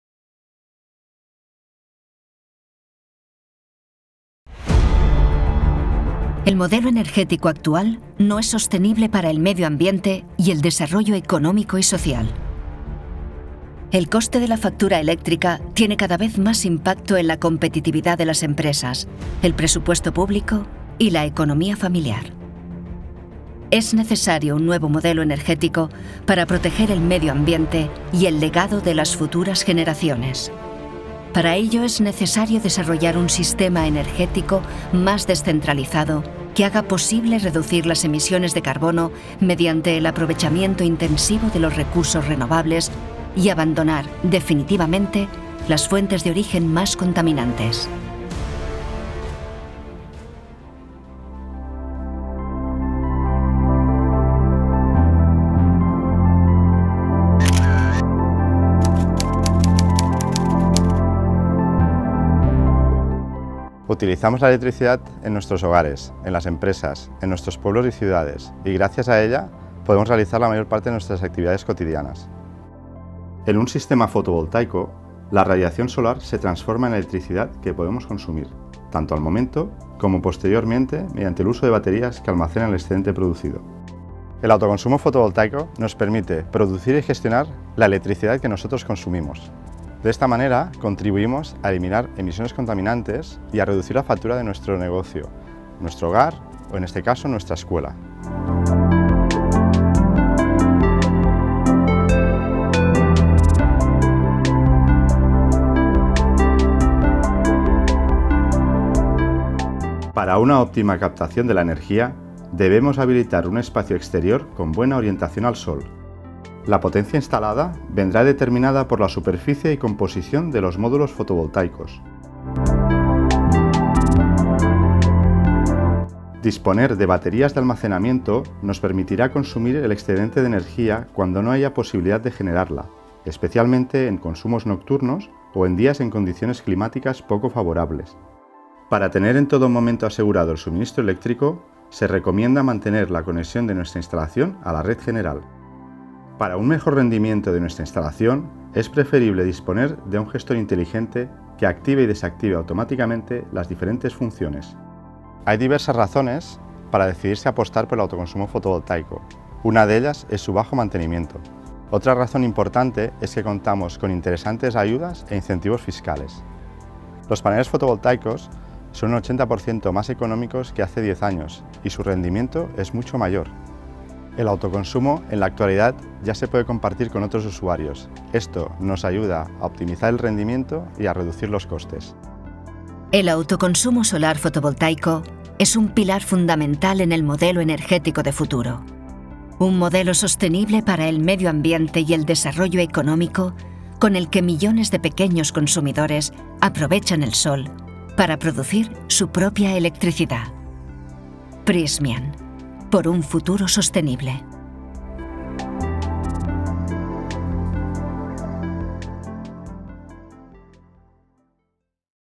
Autoconsumo Fotovoltaico. Reducir la factura eléctrica. Planeta más sostenible.
Es el momento del autoconsumo. Visualiza en este video los principales beneficios de mano del Gremio de instaladores.